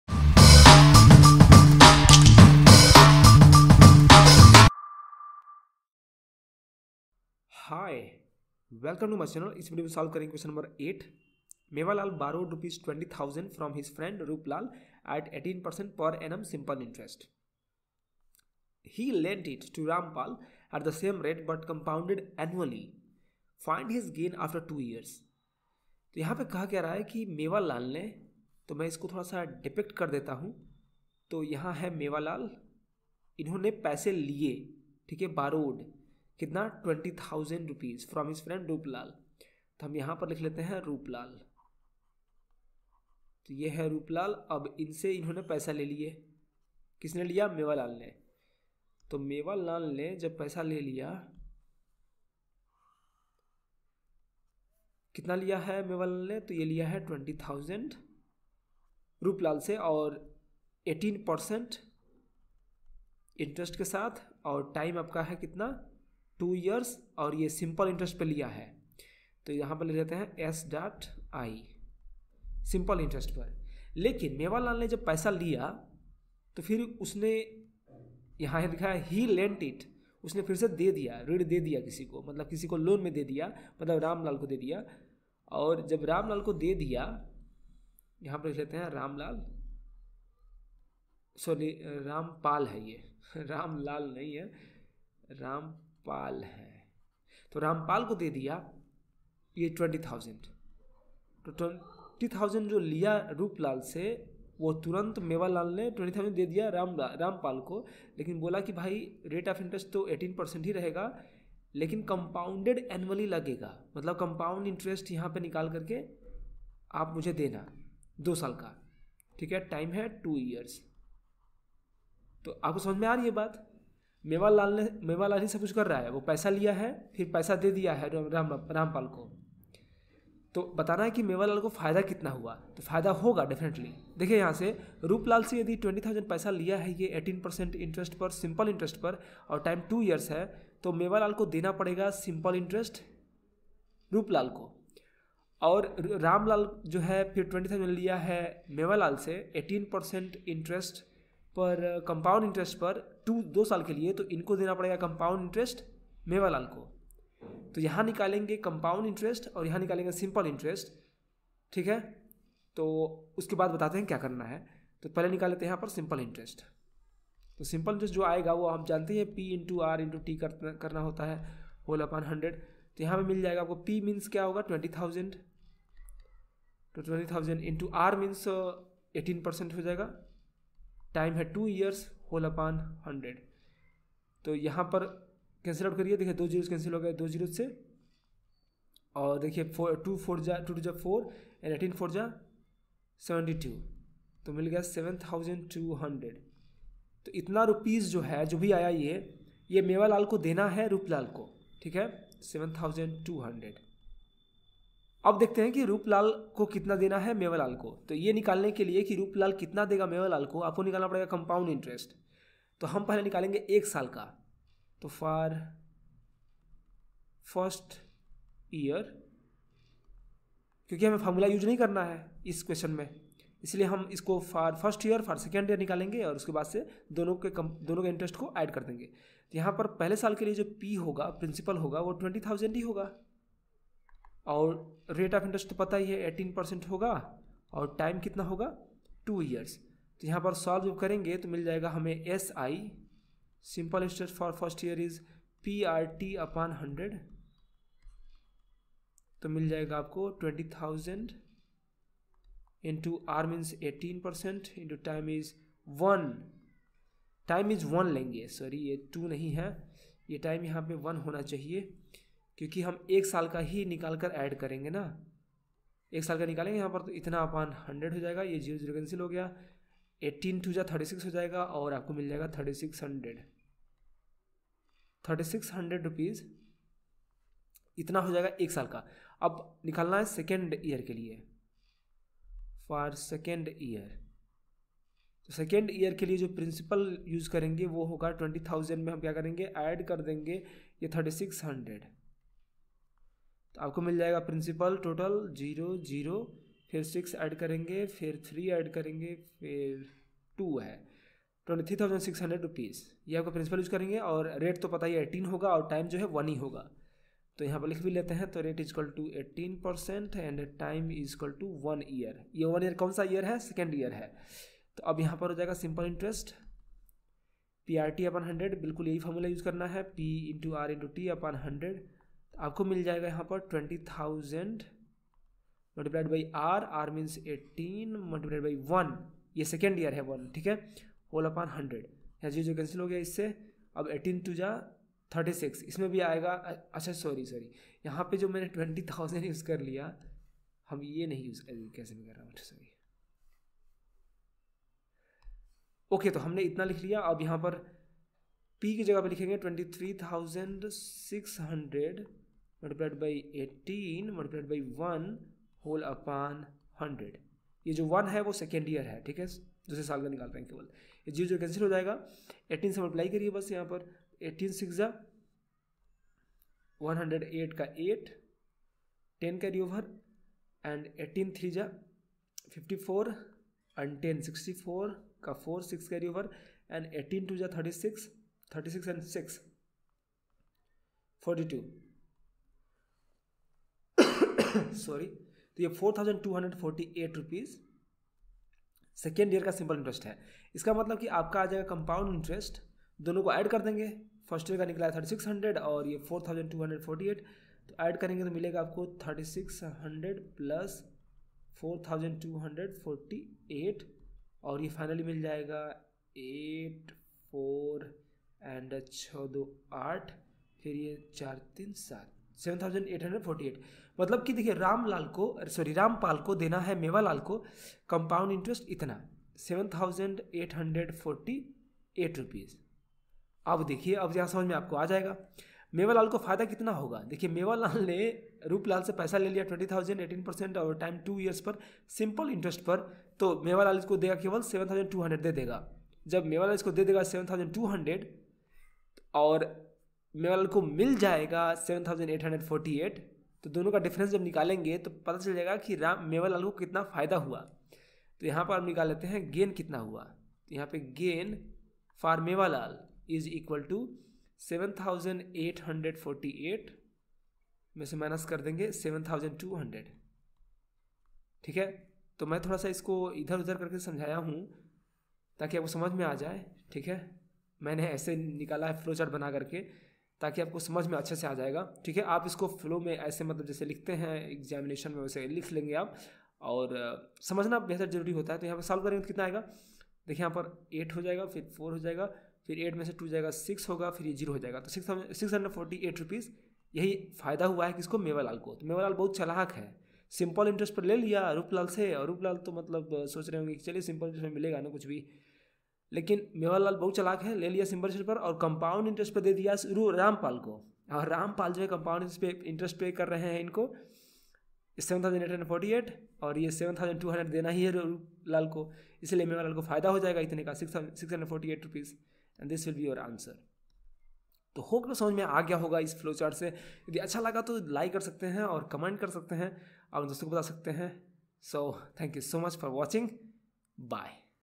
Q.8 - Exercise 14.1 - Chapter 14 - Compound Interest - RD Sharma Class 8 - Math Solutions - C.I.
In this video, question 8 from exercise 14.1 from chapter 14 Compound Interest has been fully solved with elaborate  explanation. The given question is
8. Mewalal borrowed Rs 20,000 from his friend Rooplal at 18% per annum simple interest. He lent it to Rampal at the same rate but compounded annually. Find his gain after two years.